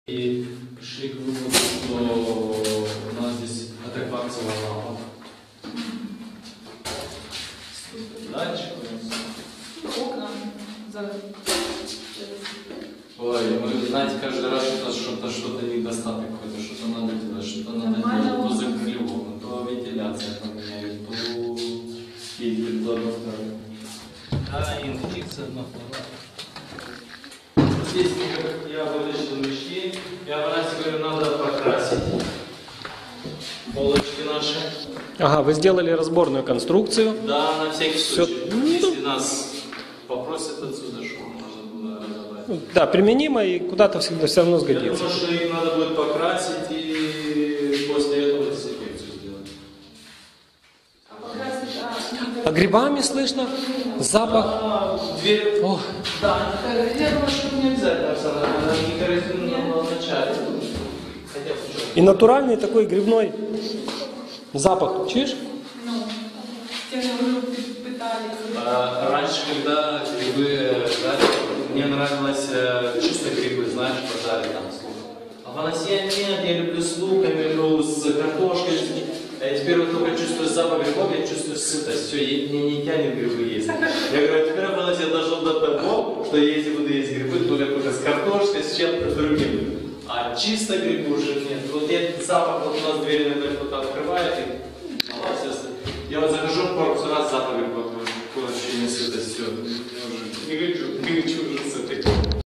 помещение для ВЫГОНКИ ГРИБОВ, подготовка камер
НАГЛЯДНЫЙ ПРИМЕР: помещение для ВЫГОНКИ ГРИБОВ. Самым главным преимуществом в выращивании грибов вешенка и шампиньйонов является возможность их выращивания в помещениях, которые больше не подходят ни для какой коммерческой деятельности: подвалы, склады, овощехранилища и др.
Большинство предпринимателей и начинают заниматься грибоводством по причине не задейсвованных пустых помещений, в которых больше нечем полезным заняться или что то другое слишком дорого. Поэтому выбирают грибы.